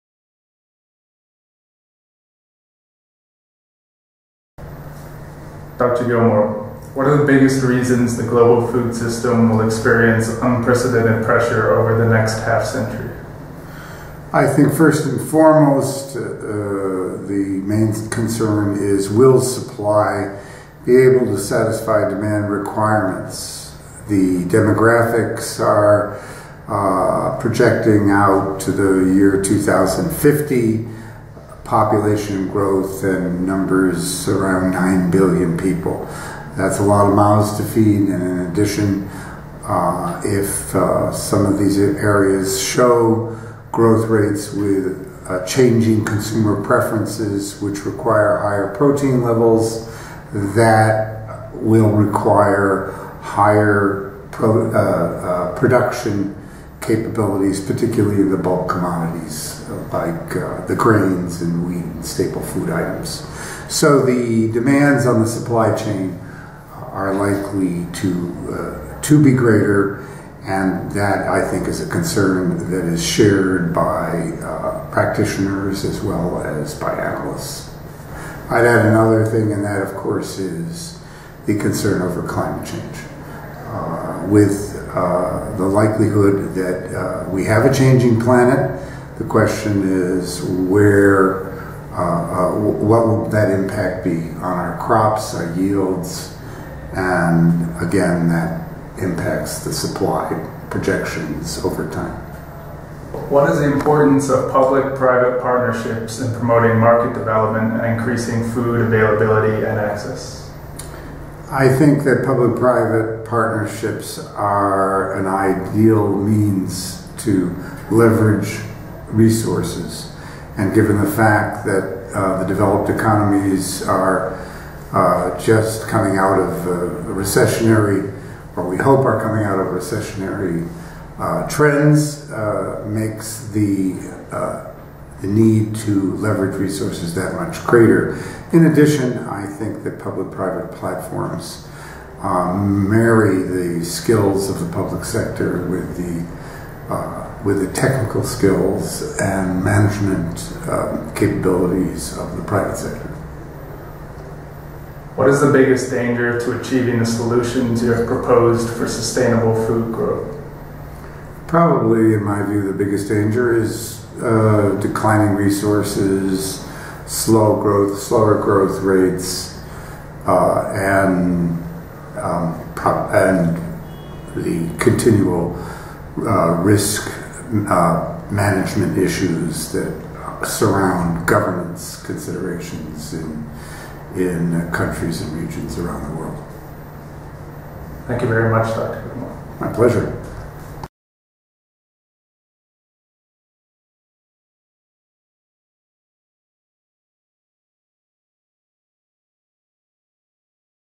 A Three-Question Interview with Richard Gilmore
The BIPR interviews Richard Gilmore,  President/CEO, GIC Group, Alexandria VA, USA; Special External Adviser to the White House and USAID for the Private Sector/Global Food Security; Managing Director of the Global Food Safety Forum (GFSF), Beijing, China, after his lecture "Global Agriculture in the 21st Century" on March 27th, 2012